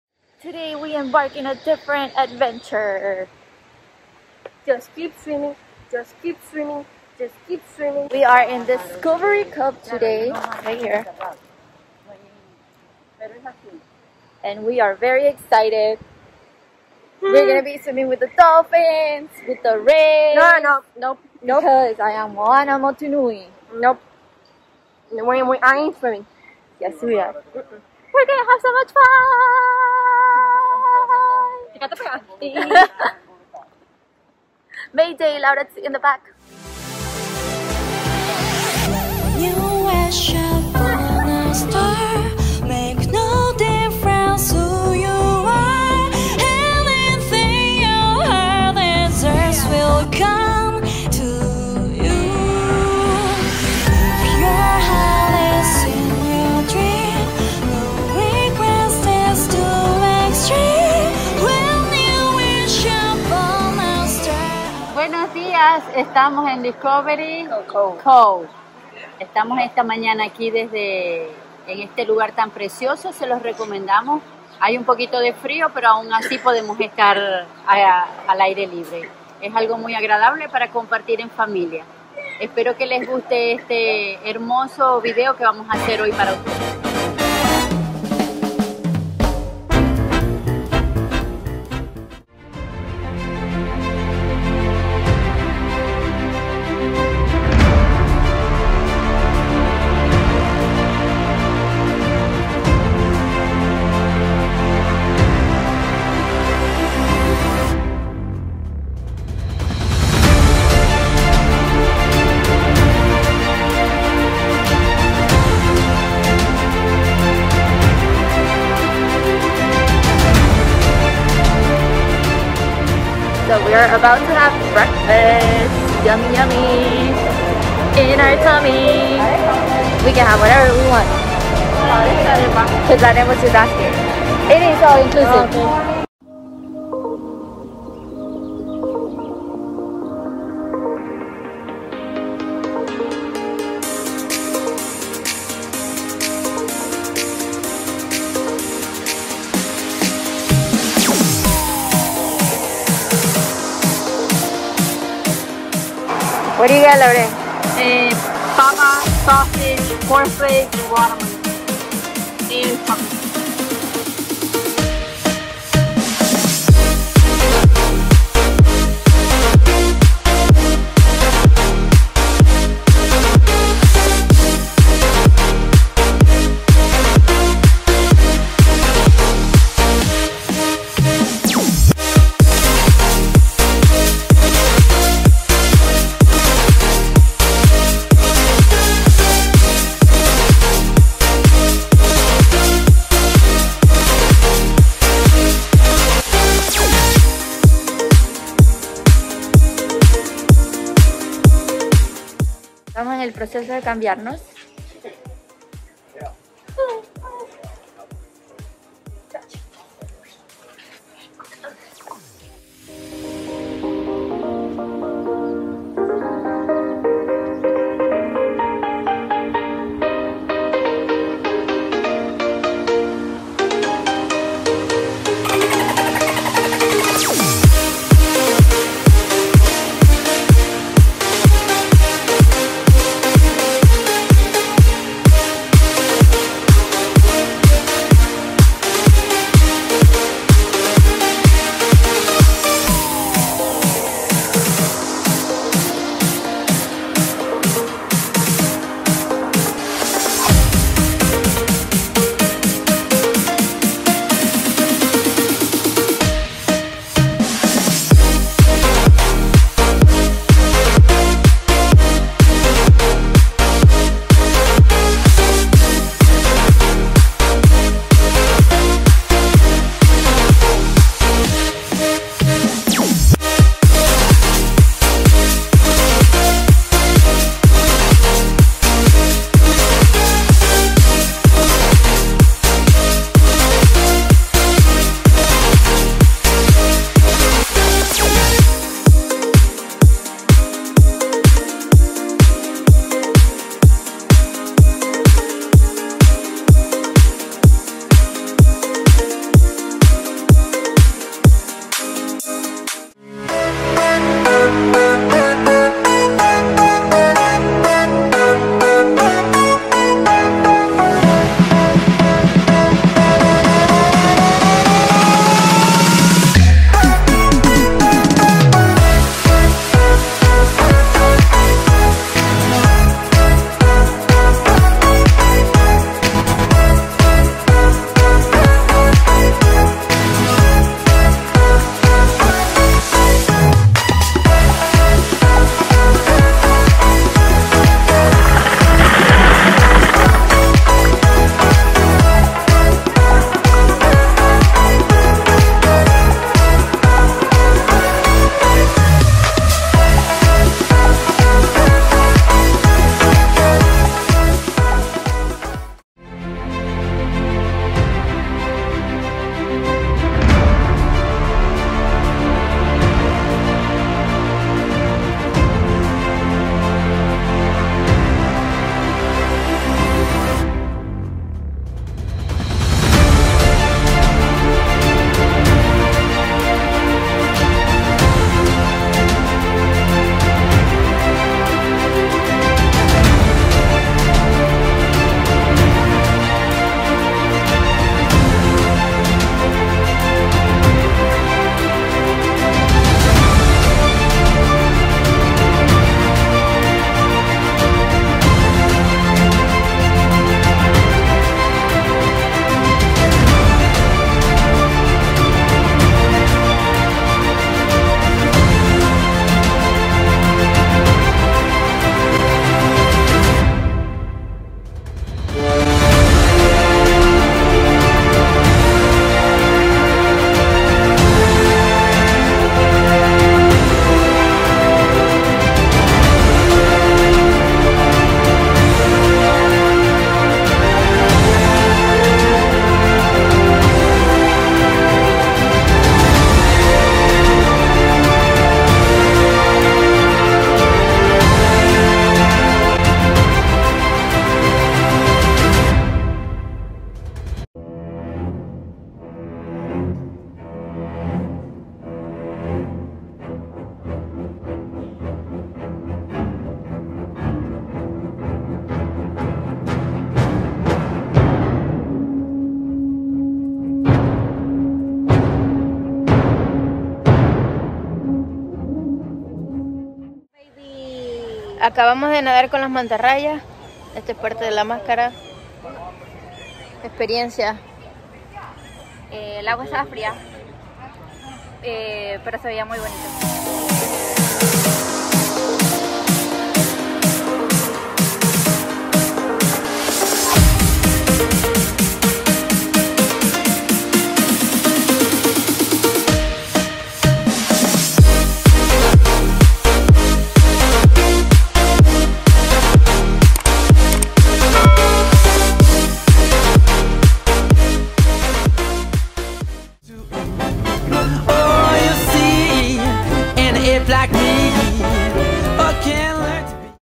Discovery Cove by Sea World
Welcome Ohana, esta semana acompananos en nuestro viaje bajo del mar. Nadamos con delfines y manta rayas en el "All Inclusive park" Discovery Cove by Sea World.

Este parque cuenta con diferentes experiencias marinas para toda la familia;  ademas incluir bebidas, comida y meriendas ilimitadas con tu ticket, y para sentirte mas en casa es un  parque acuatico que cuenta con instalaciones de baño completas para tu comodidad.

Espero disfruten este video tanto como nosotros el compartir con ustedes nuestras mágicas aventuras.